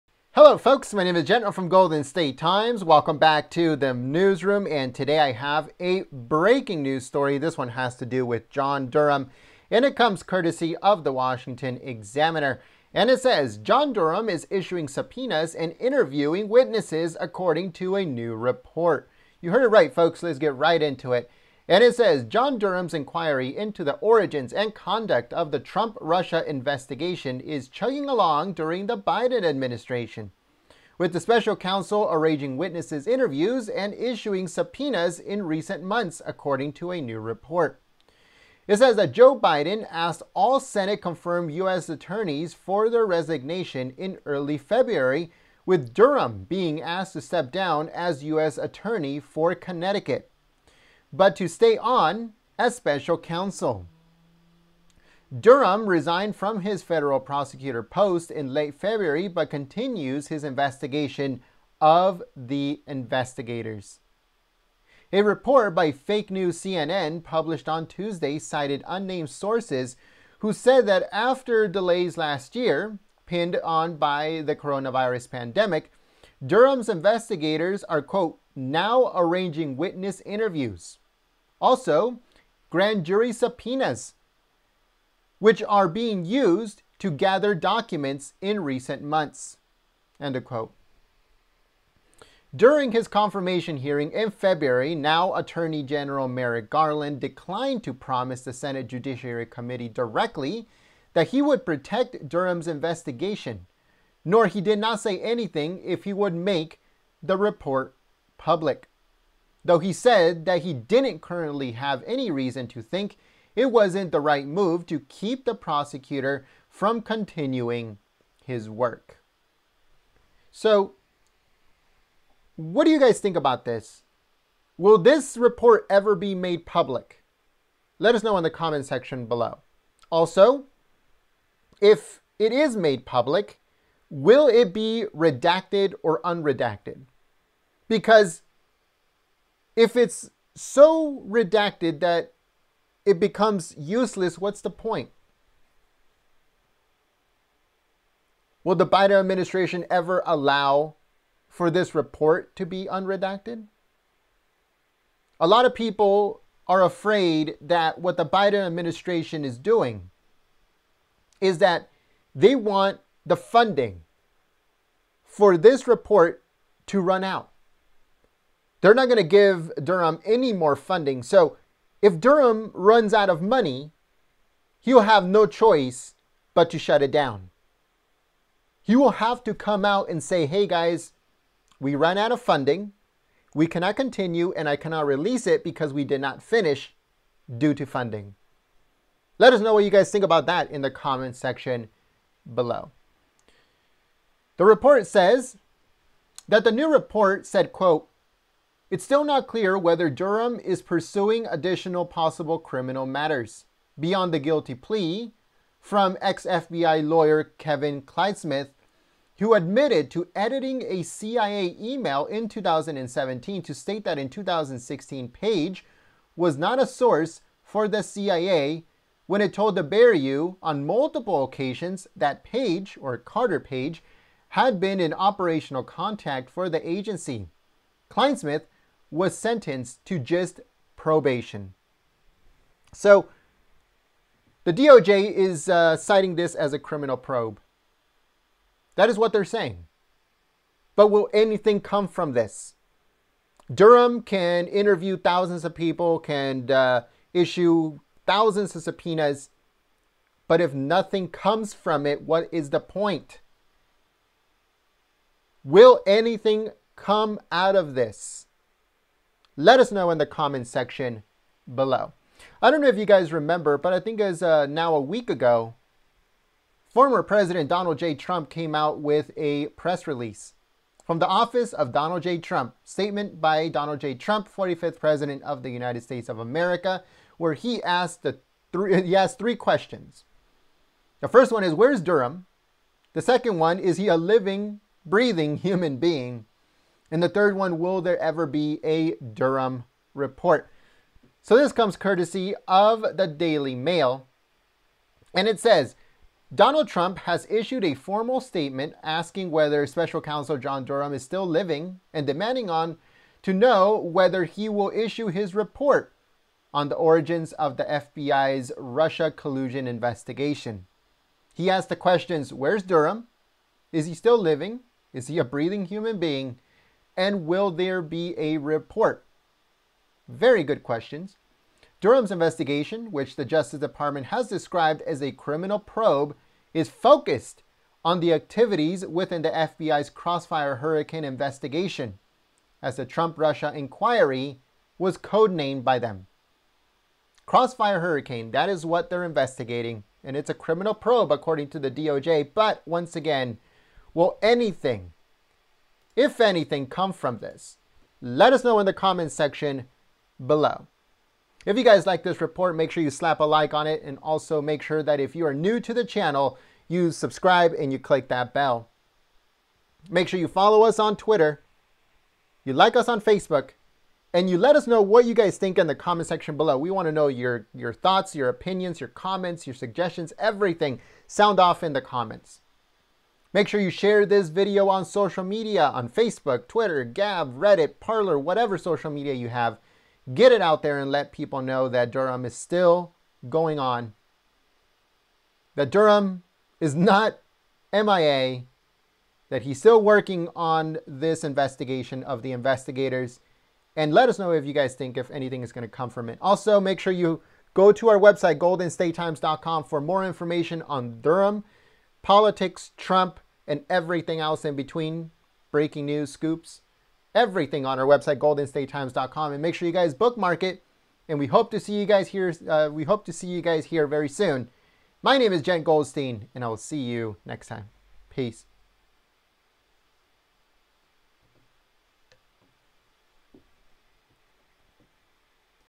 HE IS BACK: John Durham Issuing SUBPOENAS and INTERVIEWING on Crossfire Hurricane investigation!
John Durham’s inquiry into the origins and conduct of the Trump-Russia investigation is chugging along during the Biden administration, with the special counsel arranging witness interviews and issuing subpoenas in recent months, according to a new report.

Crossfire Hurricane

Donald Trump has issued a formal statement asking whether Special Counsel John Durham is still 'living' and demanding to know whether he will ever issue his report on the origins of the FBI's Russia collusion investigation.